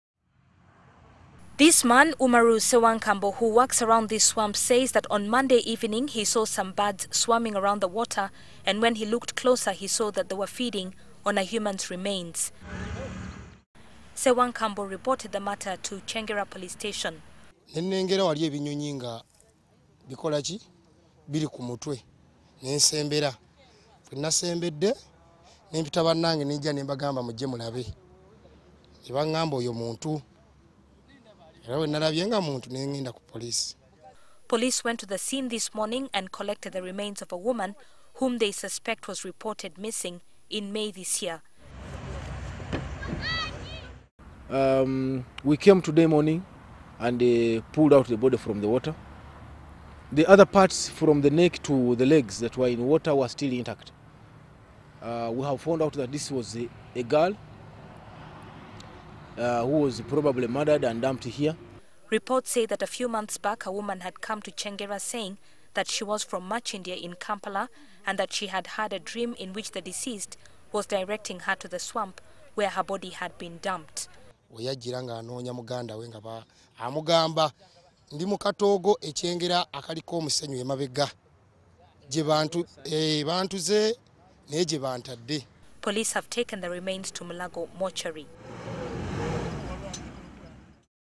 Body of missing woman found in Kyengera swamp
Police in Kyengera have discovered the body of a woman who went missing in May this year. This after locals selling sand in the Kitoogo area noticed a swarm of birds around the swamp  and on getting closer, saw a human’s skull.   Police have taken the remains to Mulago mortuary.